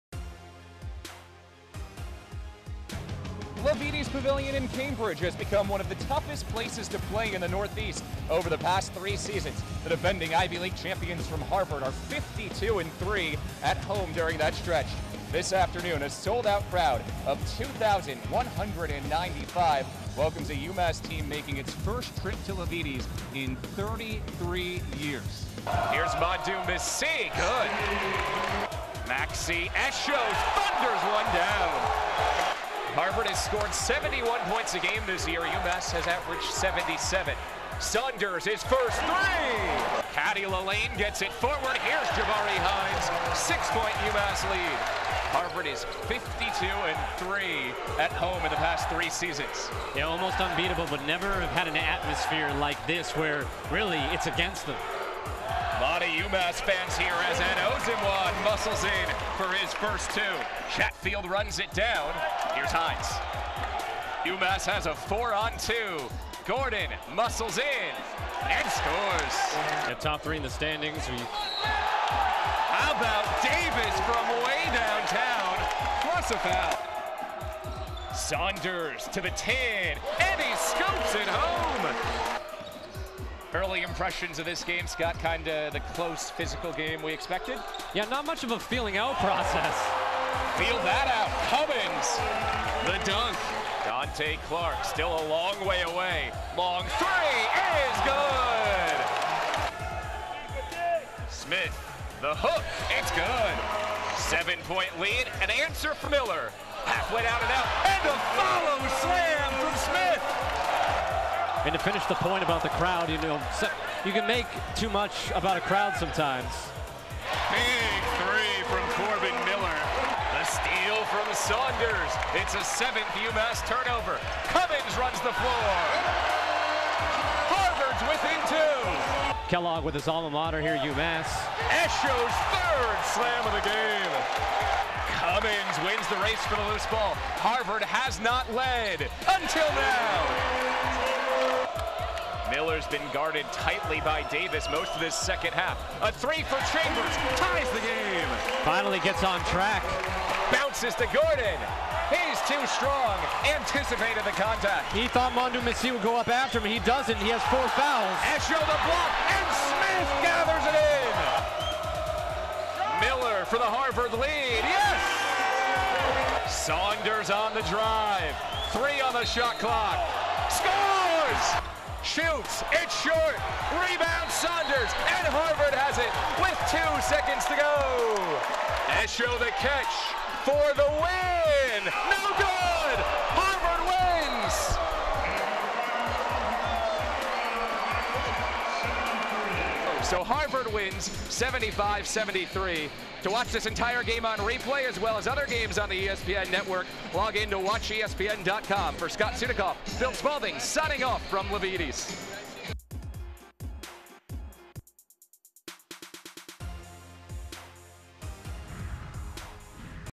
Game Recap: Strong Second Half Lifts Men's Basketball Over UMass, 75-73
The Crimson defeated UMass in front of a sold-out crowd thanks to a strong second-half comeback.